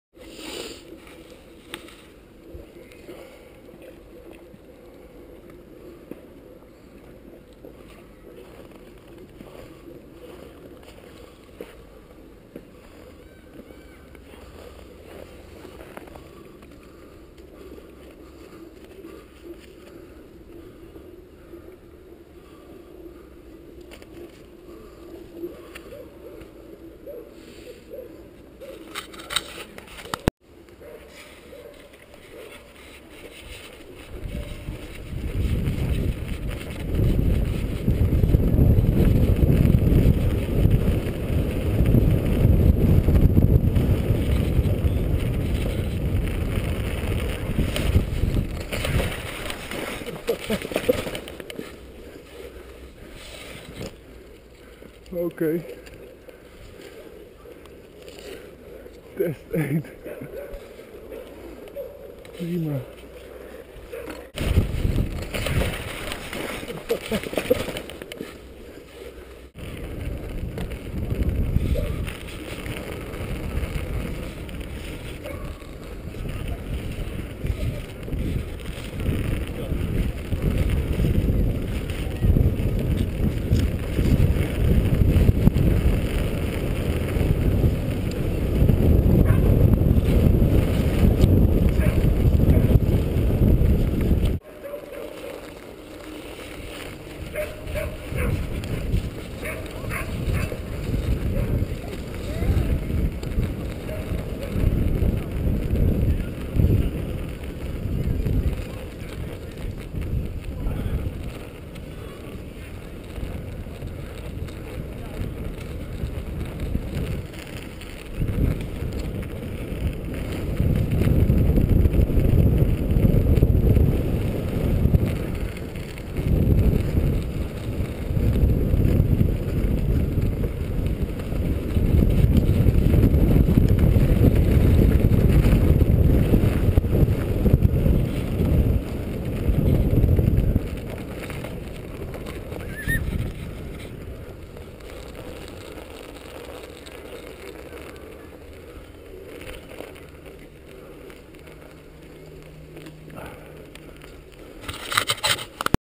Riding my bike on ice
Riding my bike on ice in Arnhem.
Build my own spiked ice tires with normal road race tires.